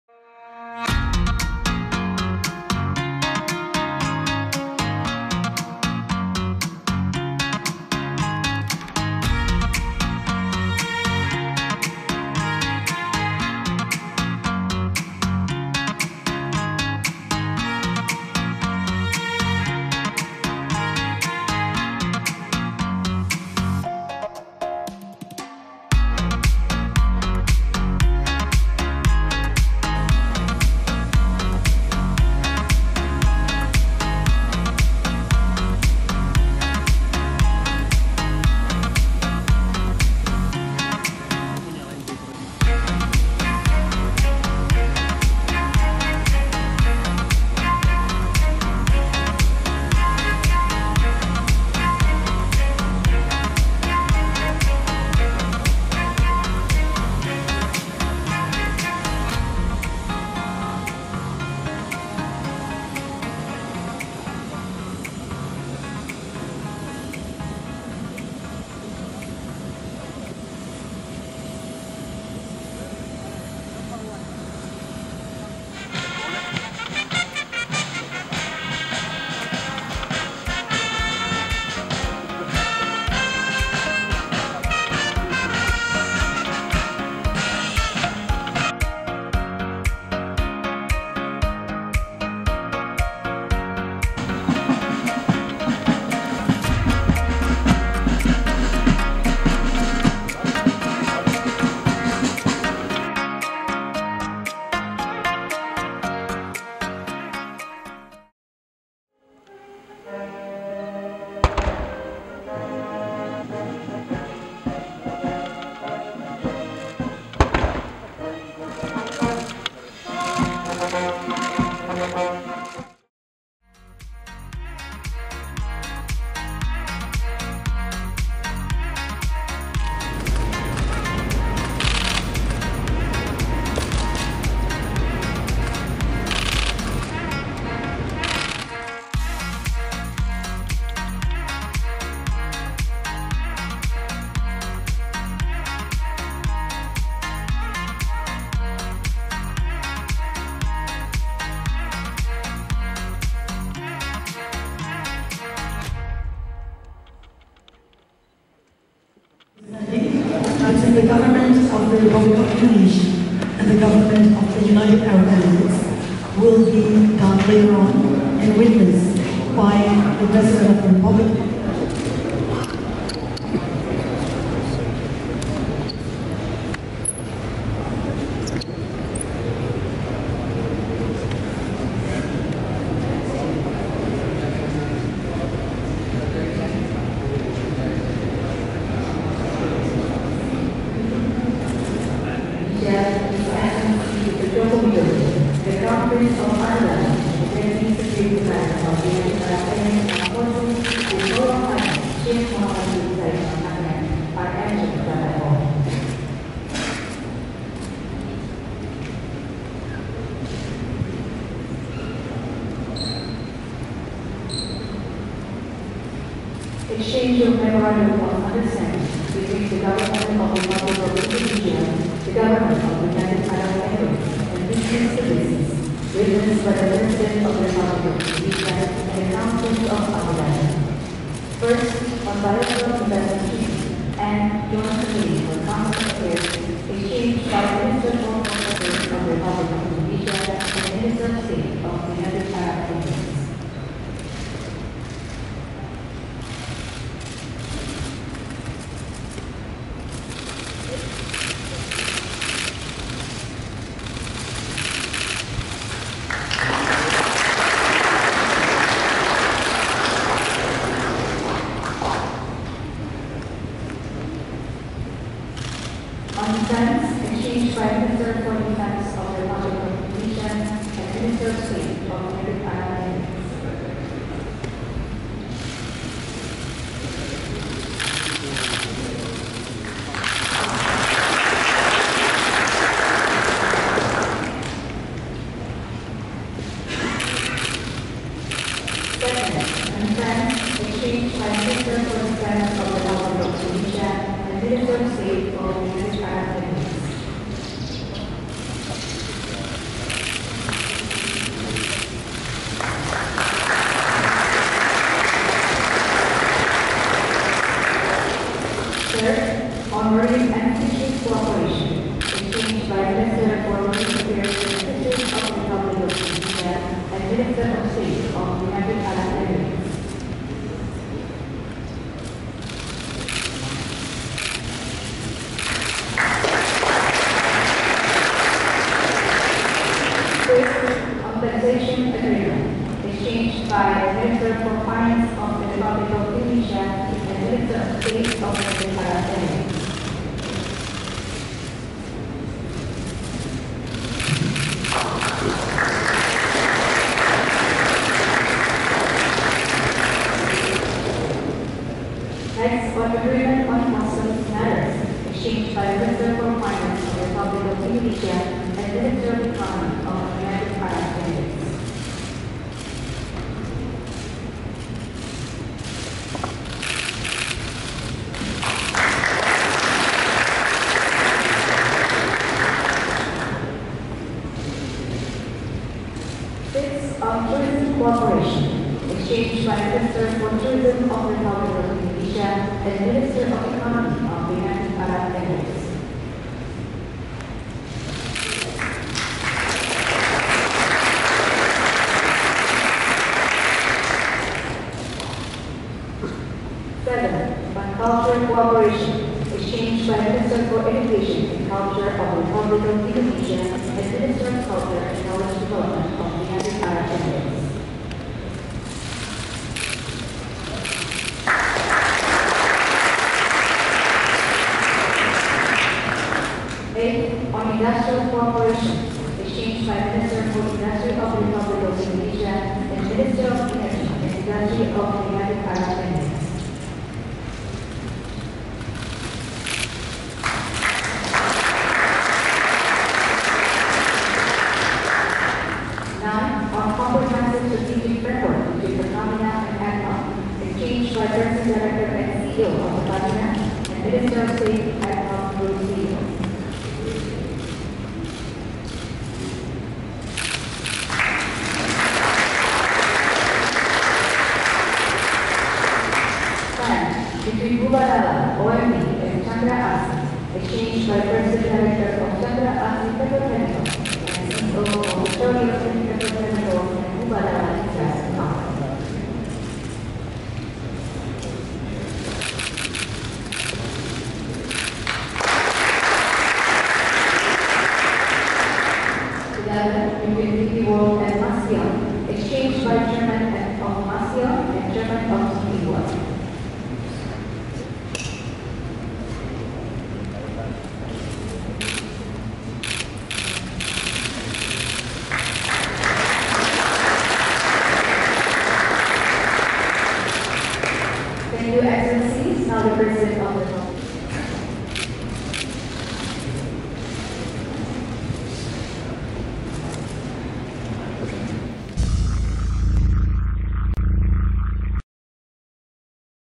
Kunjungan Kenegaraan Putra Mahkota Abu Dhabi, 24 Juli 2019
Presiden Joko Widodo menyambut Kunjungan Kenegaraan Putra Mahkota Abu Dhabi/Wakil Panglima Tertinggi Angkatan Bersenjata Uni Emirat Arab (UEA), Sheikh Mohamed Bin Zayed Al Nahyan, Rabu, 24 Juli 2019.

Rangkaian acara penyambutan dimulai dari Bandara Internasional Soekarno Hatta, Tangerang, Banten, Rabu (24/7) pagi. Kemudian dilanjutkan dengan upacara kenegaraan di Istana Kepresidenan Bogor.